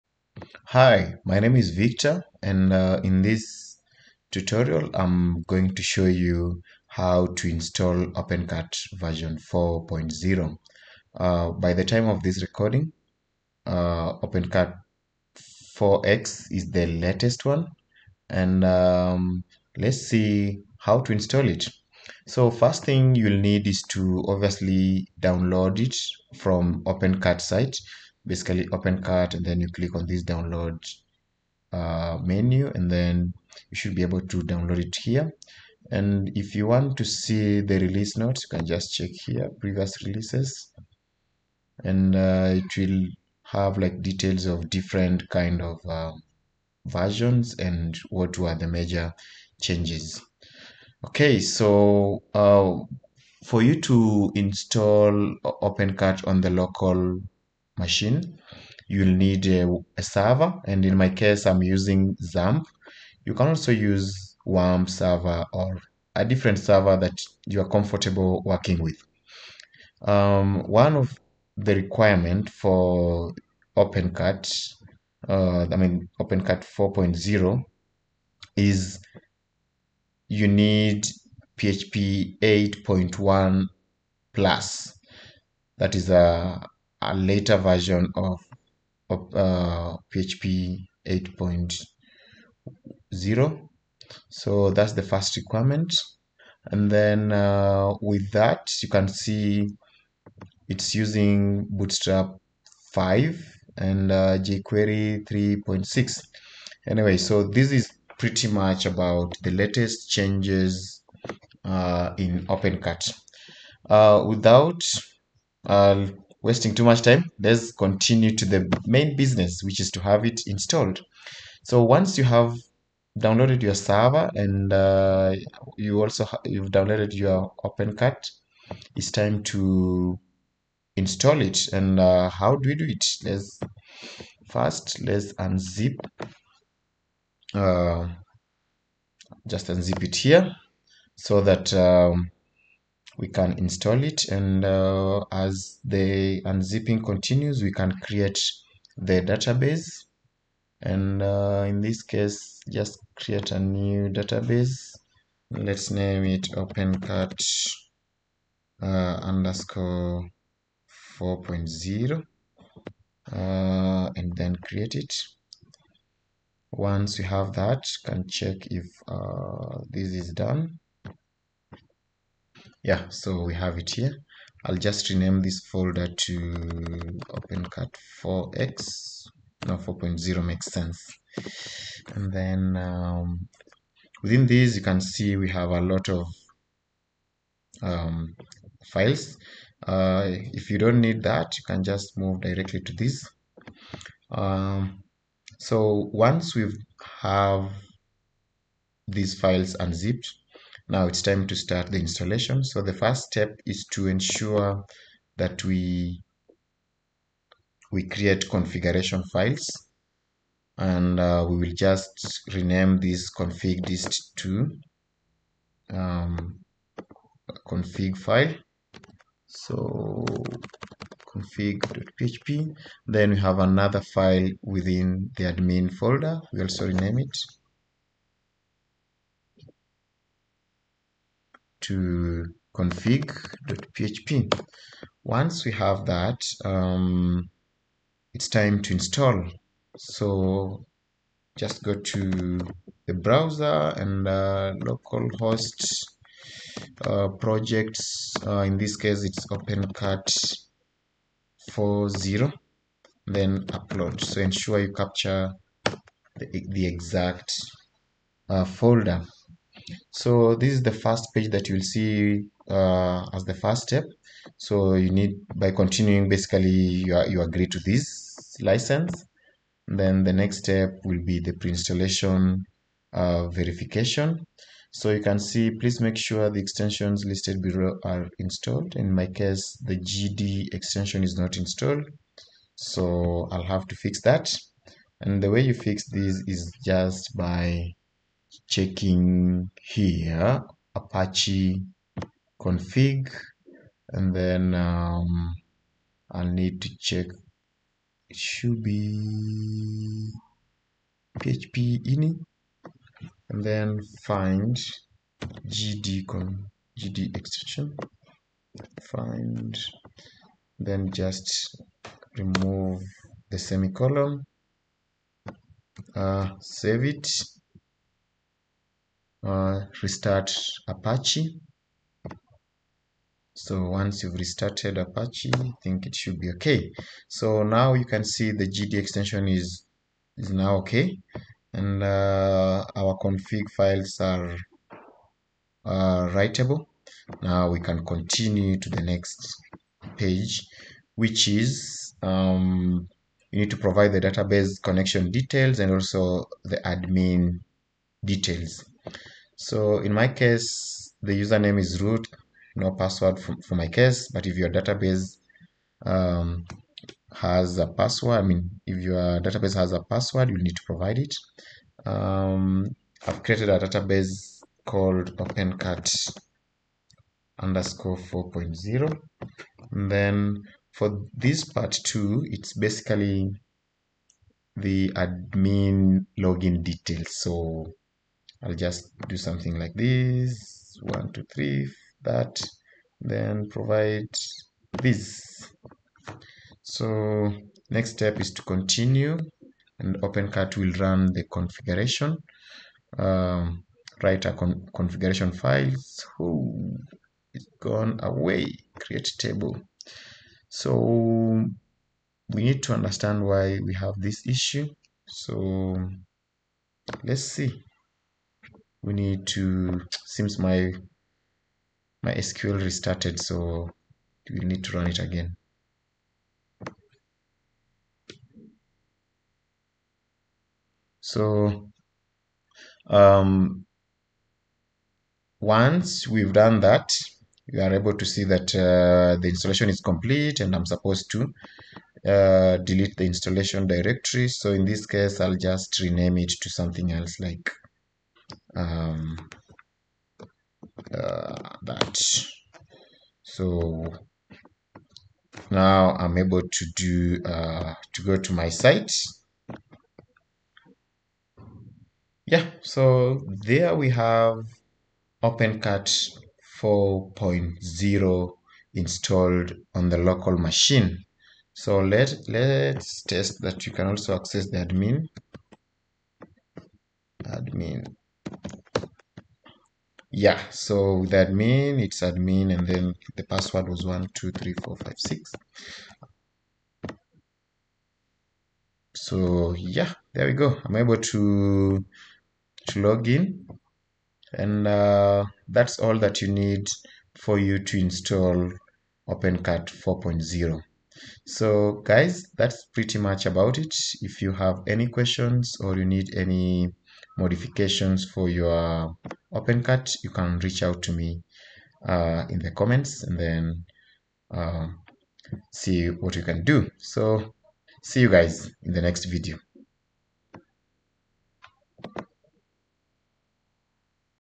How to install OpenCart 4.0
This video shows you how to install OpenCart 4.0.0.0  on your local machine.